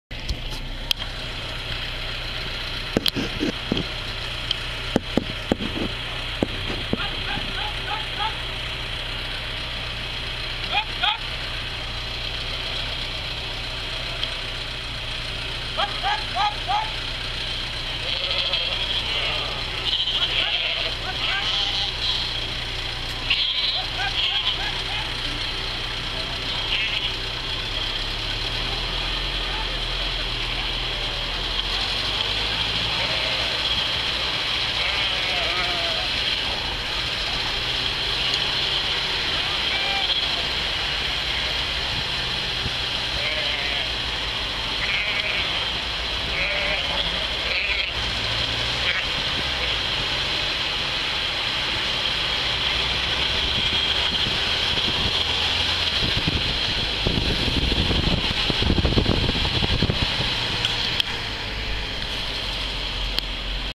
Niemiecka Policja eskortuje owce przez most
powódź 2013 Elba Magdeburg